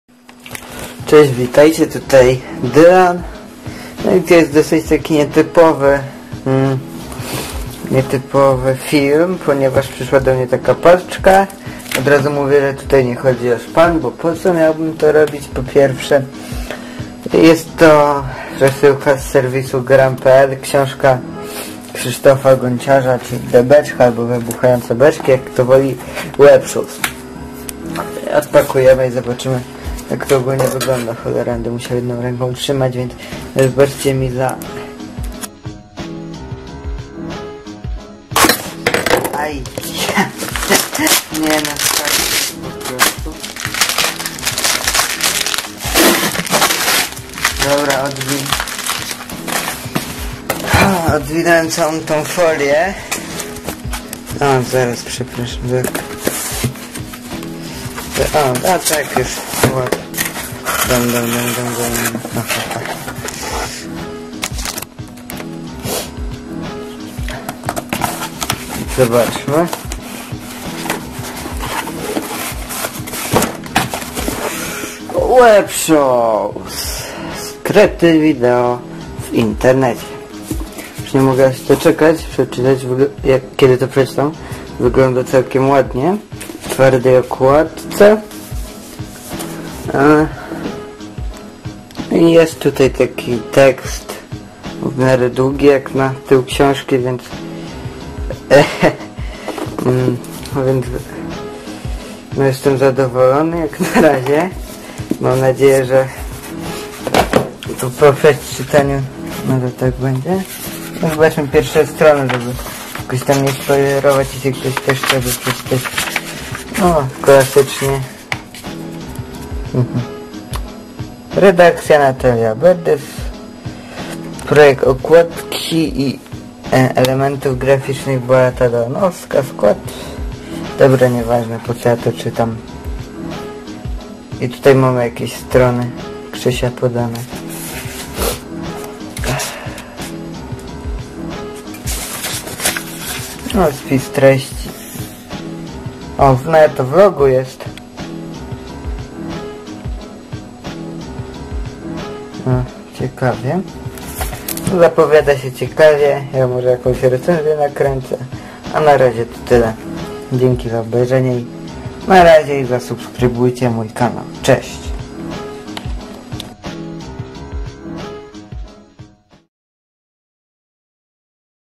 Ja z kamerą - Unboxing książki Webshows - Krzysztof Gonciarz
Uwaga ten film nie ma na celu:
1. Szpanu telefonem, bo wiem, że takie komentarze, też mogą się pojawić
2. Promocji kosztem Krzysia.
3. Szpanu z posiadania tej książki
4. I innej formy szpanu, którą hejterzy mogą sobie uroić.
Pozdrawiam i życzę przyjemnego oglądania :)